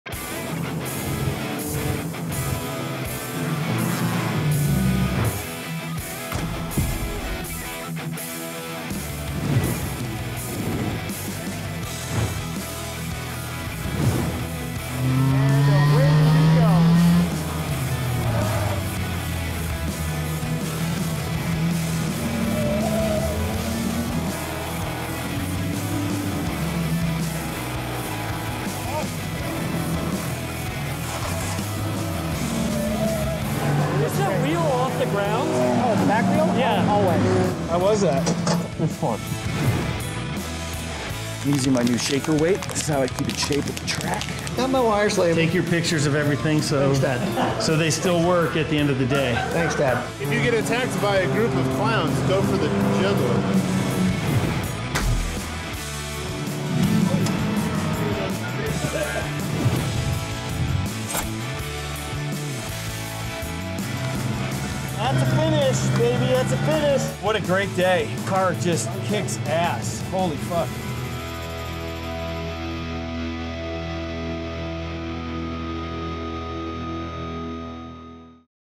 Wrenching to Race Trailer
Race fans, you asked for it, now you have it! Whether that's a good thing or a bad thing, it will live on forever on youtube! Take a super quick look at the Brooklyn Bomb Squad in action. Enjoy!